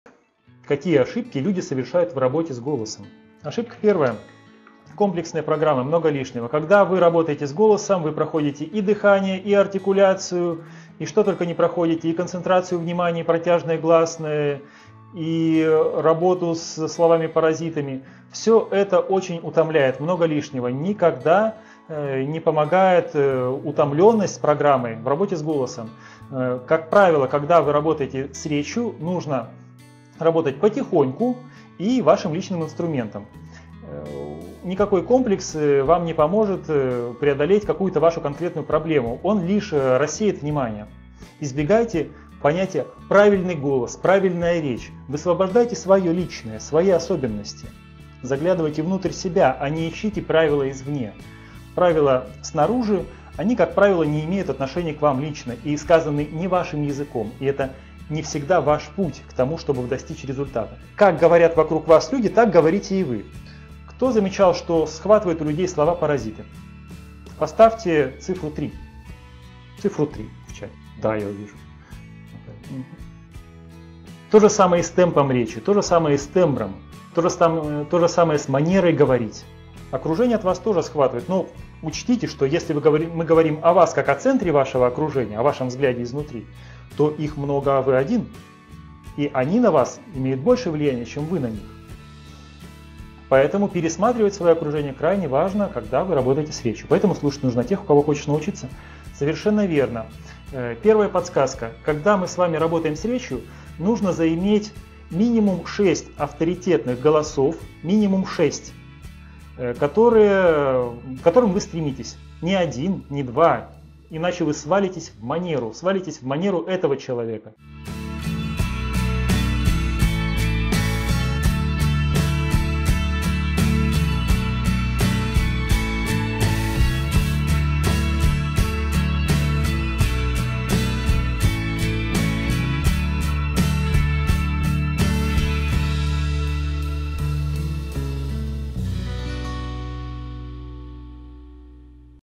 3 ошибки, которые портят работу с речью и голосом
Какие ошибки люди совершают в работе с голосом.
Ошибка первая: Много лишнего.
Когда вы работаете с голосом, вы проходите и дыхание и артикуляцию и концентрацию внимания и протяжные гласные и работу со словами-паразитами. Все это очень утомляет. Много лишнего. Никогда не помогает утомленность программой в работе с голосом. Как правило, когда вы работаете с речью, нужно работать потихоньку и вашим личным инструментом. Никакой комплекс вам не поможет преодолеть вашу конкретную проблему. Он лишь рассеет внимание.

Ошибка вторая: Правильность.
Избегайте понятия "Правильный голос", "Правильная речь". Высвобождайте свое личное, свои особенности. Заглядывайте внутрь себя, а не ищите правила извне. Правила снаружи, как правило, не имеют отношения к вам лично и сказаны не вашим языком. И это не всегда ваш путь к тому, чтобы достичь результата.

Ошибка третья: Окружение.
Как говорят вокруг вас люди, так говорите и вы. Кто замечал, что схватывает у людей слова-паразиты - поставьте цифру "3" в чате. Тоже самое и с темпом речи, то же самое и с тембром, с манерой говорить. Окружение от вас тоже схватывает. Но учтите, что если мы говорим о вас как о центре вашего окружения, о вашем взгляде изнутри, то их много, а вы один. и они на вас имеют больше влияния, чем вы на них. Поэтому пересматривать свое окружение крайне важно, когда вы работаете с речью.